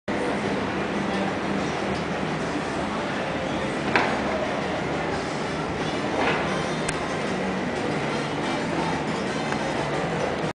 almorzando en salvador de bahia
Los profesores de historia Angel Popolizio, Julio Djenderedjian y Juan Lázara comparten un almuerzo en un bello restaurante frente a una vista panorámica de Salvador de Bahia